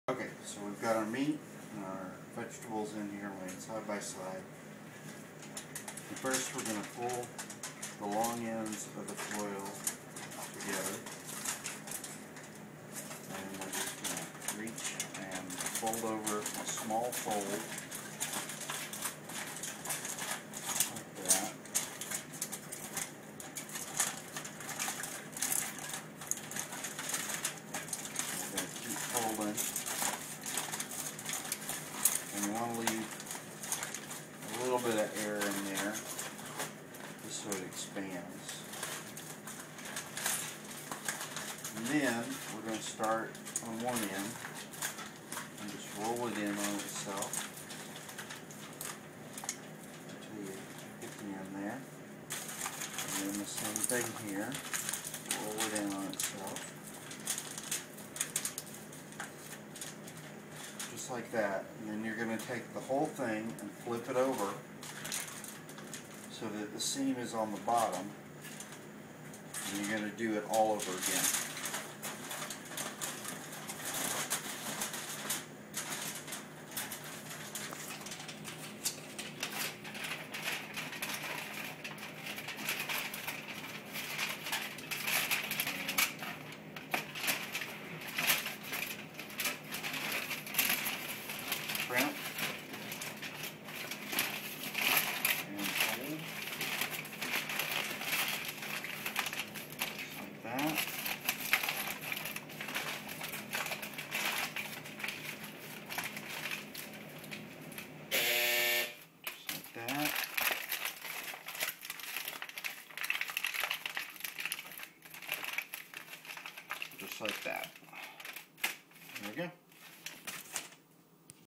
Foil burgers
How to properly wrap up foil burgers.